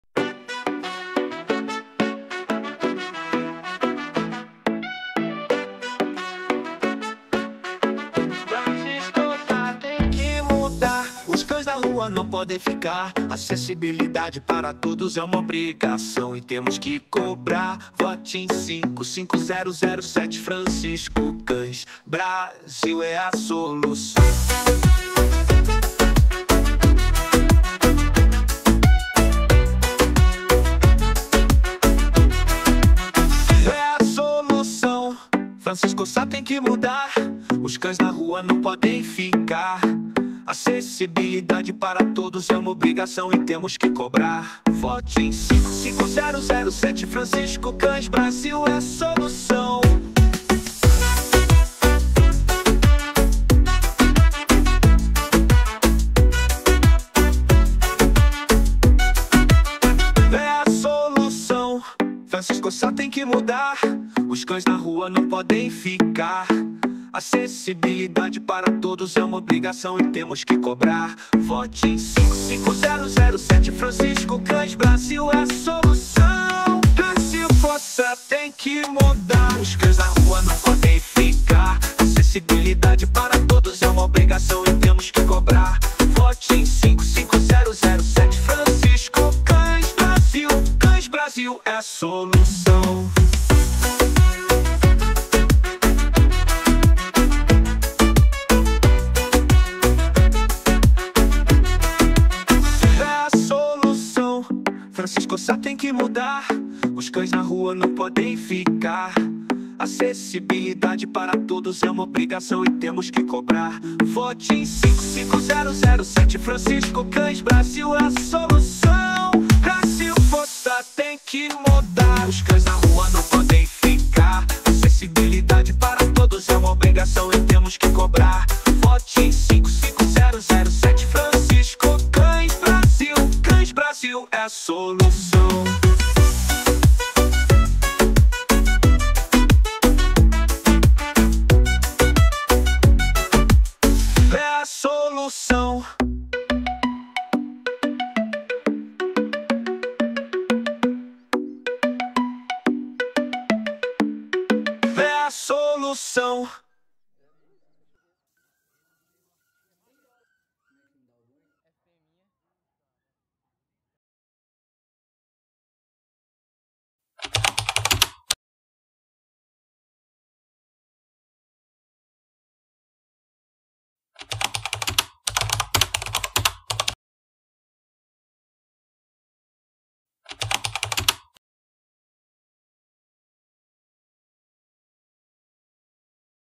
Francisco Cães Brasil Agora é a Hora e Mudar
Agora é a hoje, tomara que dê certo pois precisamos muito Resolver esse problema que só cresce em Francisco Sá MG a cada dia que passa, então já que nunca entrou ninguém para focar em Resolver essa Situação, e eu já mexo com isso sempre a anos, então bora lá né rsr vamos correr atrás de Resolver isso, lembrando que cachorro só é atropelados na rua se estiverem na rua, cachorra só pega cria e pare nas ruas se não forem castradas, e como aqui não tem clinica e nem canil municipal e nem ong, na verdade é um descaso não tem e nada sobre esse assunto, ai tudo fica mais difícil, e quando chega época policia muitos prometem muita coisa e eu já estou cansado disso, é como já falei algumas vezes no youtube cães Brasil, " Falar até papagaio fala, agora fazer rsr? ai é outros quinhentos". chega disso, então quem apoia e quer Resolver essa situação de cães de rua, e acessibilidade para cadeirantes, mães com carrinho de bebê, pessoal com sequelas de avc etc, todos precisam de acessibilidade para se locomover com segurança, além de faixa de pedestre etc, coisas simples que fazem muita diferença, quem quer apoia e me dar uma chance de apresentar e correr atrás de mudanças onde gostaria muito também da participação da população falando oque gostaria de ver aqui na cidade, oque Francisco Sá precisa para melhorar, ideias e sugestões serão sempre bem vindas, assim fica mais fácil da população ter voz, sou simples e sei oque bastante gente aqui passa, vamos levar para a Câmera Municipal oque realmente importa para ser Resolvido, esse é meu objetivo e conto com vocês, mesmo que muitos não podem comentar aqui e nem curtir e nem nada, devido questão de politica mesmo e sei como é isso, então oque realmente conta e o voto de confiança 55007 basta votar e pronto, sua parte você já vez e dando certo deixa que o resto eu faço, e se eu não conseguir então vou ir atrás de quem tem poder para fazer, o importante é não ficar parado, e conto com você vote 55007 a sua participação vai ajudar muito a resolver oque nunca ninguém resolveu aqui em Francisco Sá, esse é meu compromisso e que Deus abençoe a todos.